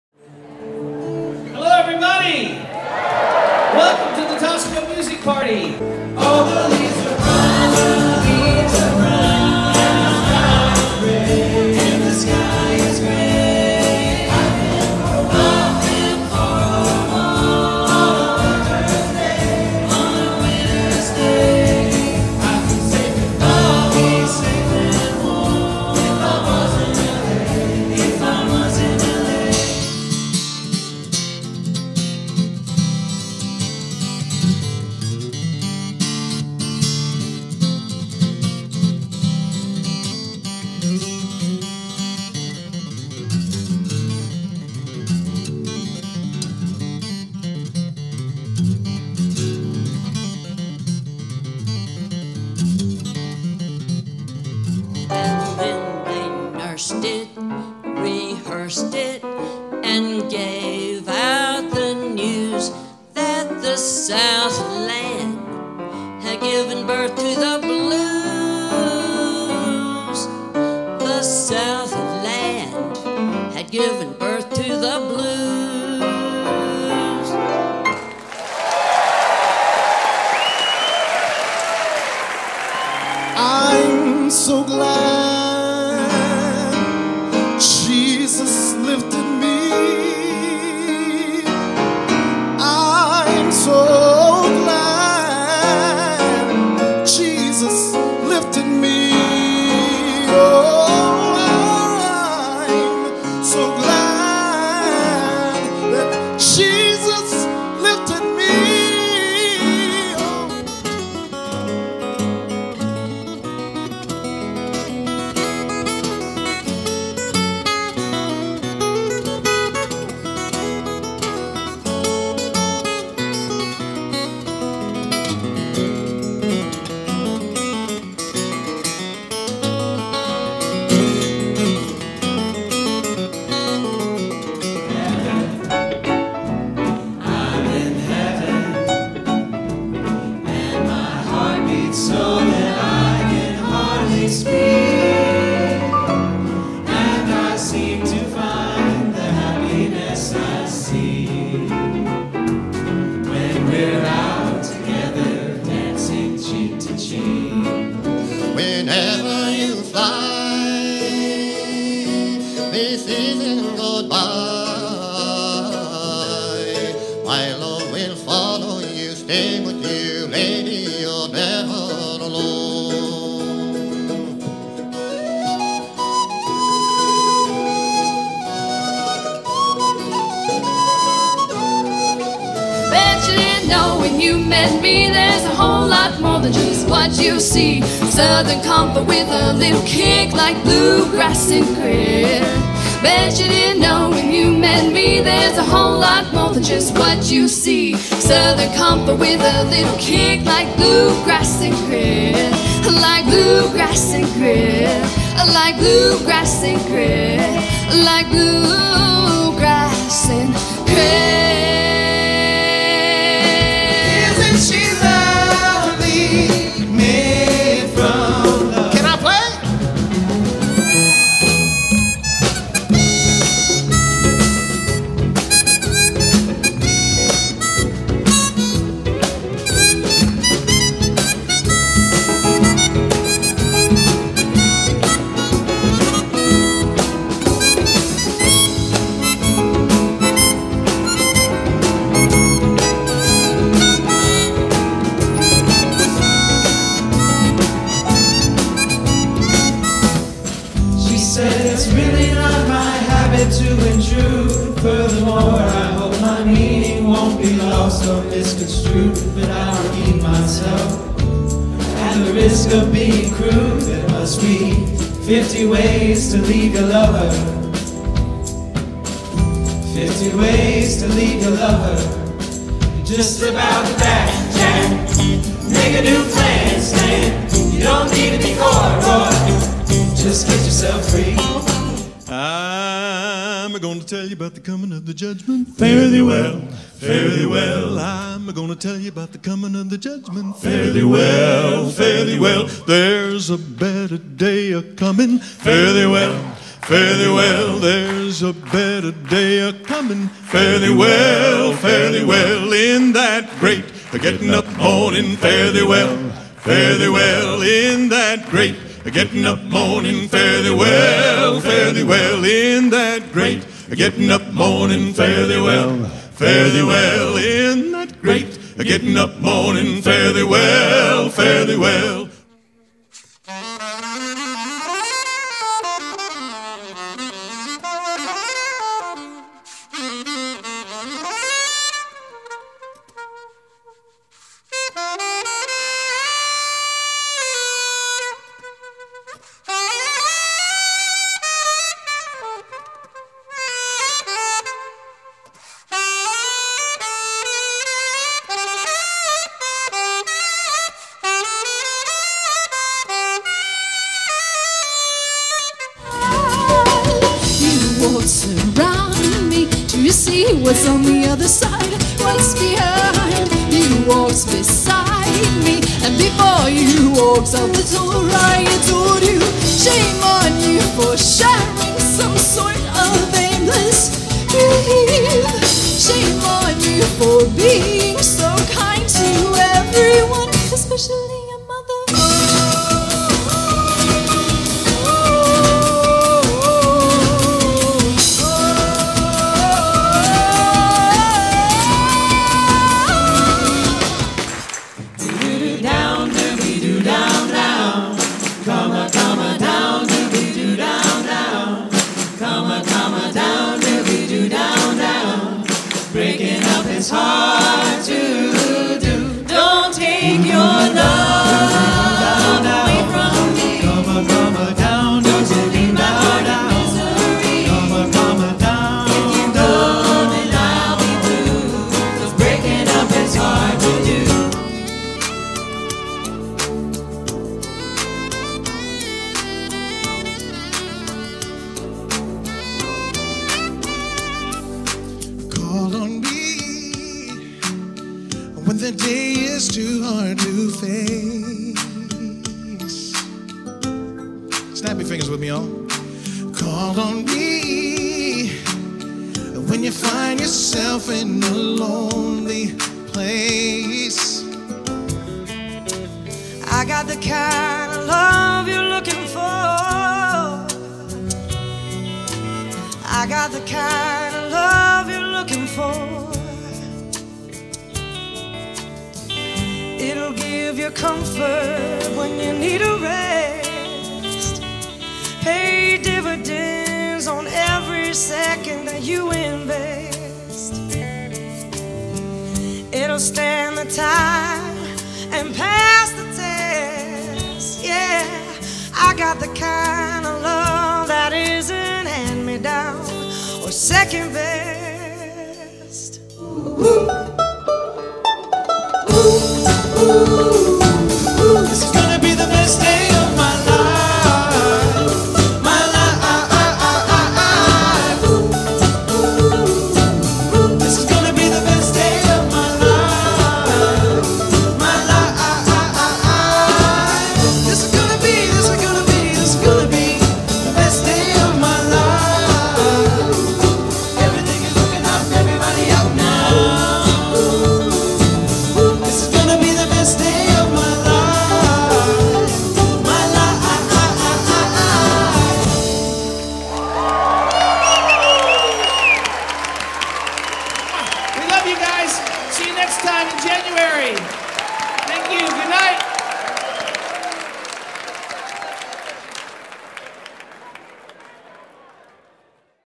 Tosco Music Party Highlights from Sept 20, 2014
The Tosco Music Party is held three times a year at the Knight Theater in Charlotte, NC and features a variety of local, regional and national music performers.